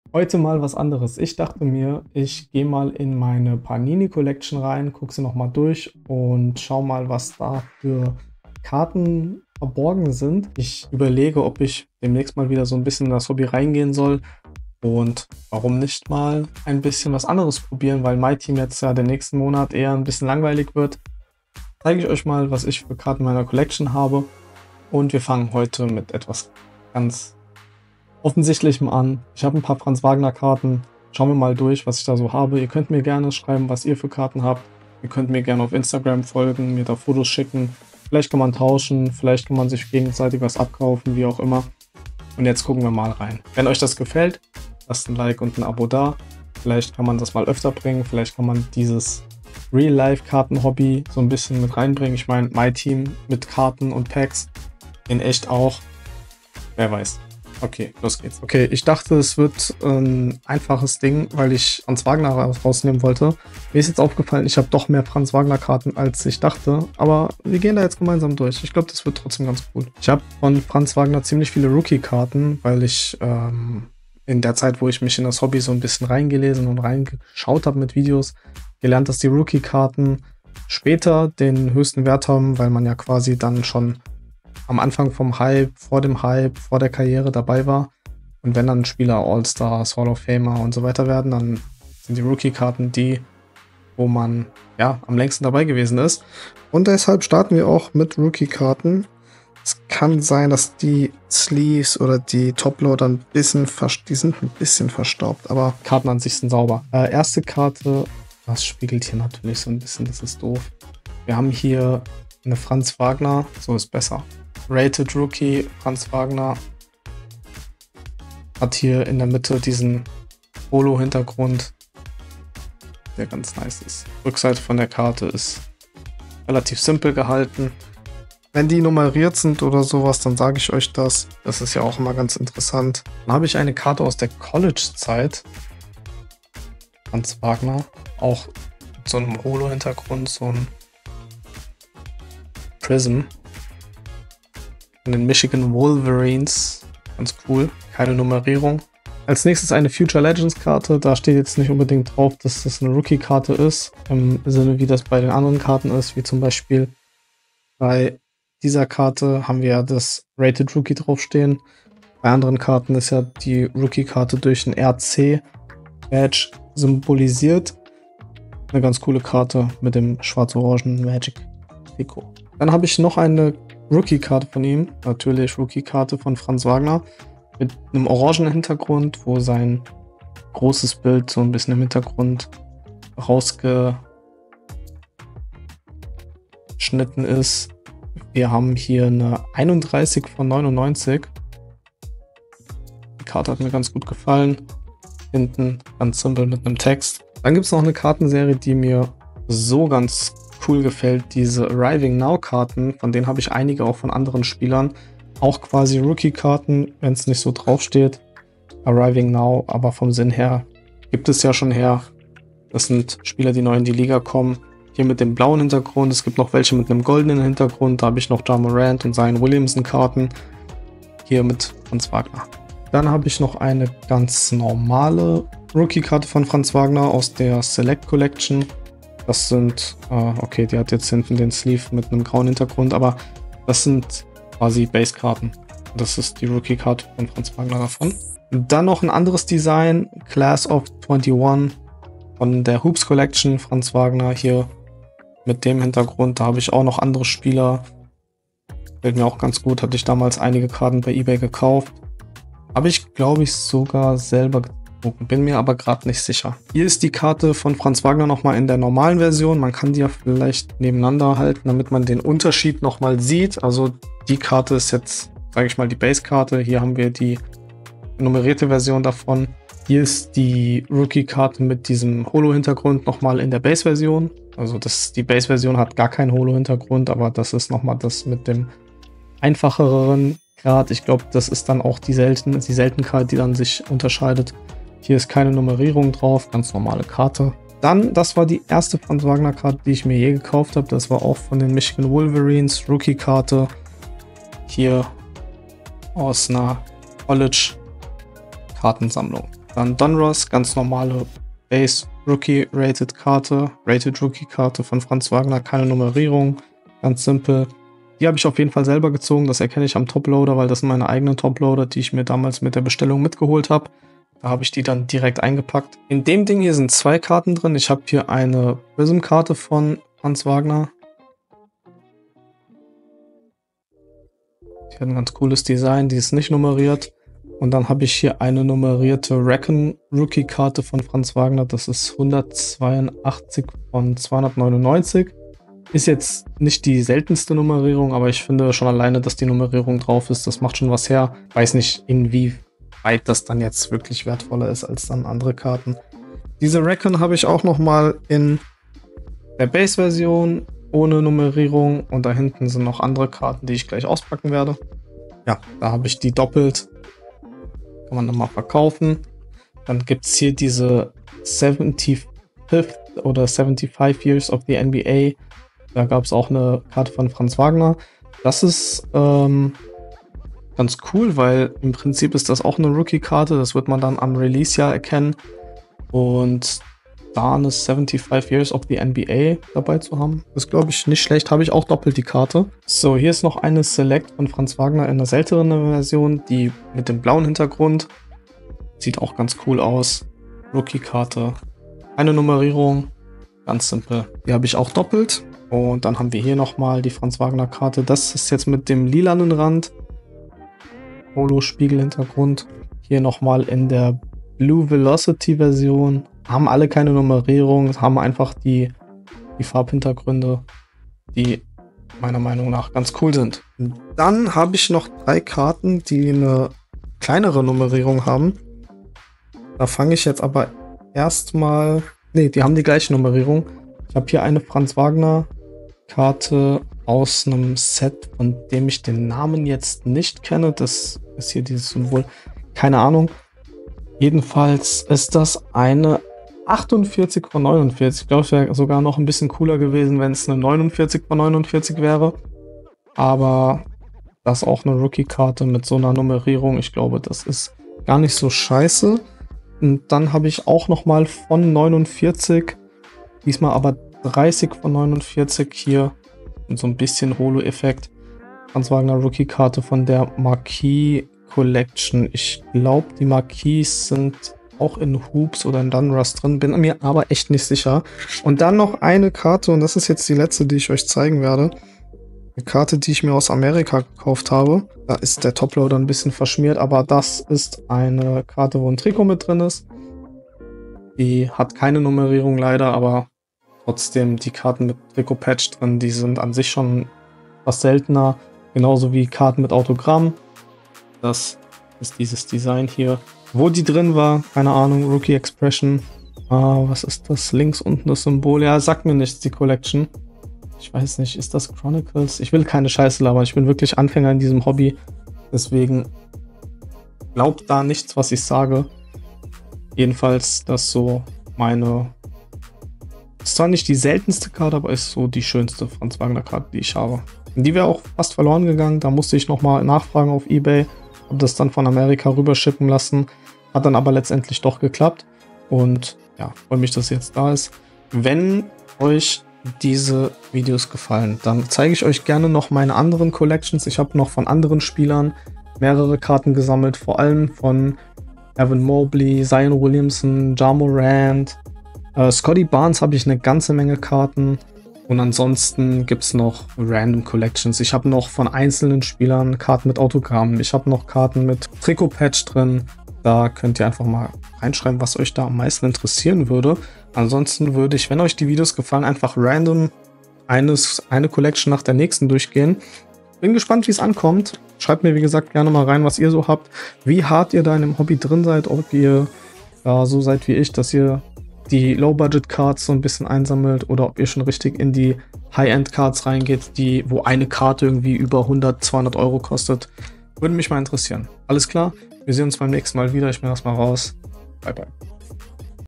Meine Franz Wagner Sammlung | Panini NBA Trading Cards
Heute zeige ich euch mal etwas anderes, nämlich echte Karten, die ihren Wert nicht verlieren, wenn eine neue Serie raus gebracht wird :)

Günstigere PSN Karten? HIER!
PSN 50€ = 41,79€
20€ = 17,09€

Folge dem Kanal für alle Updates und Streams.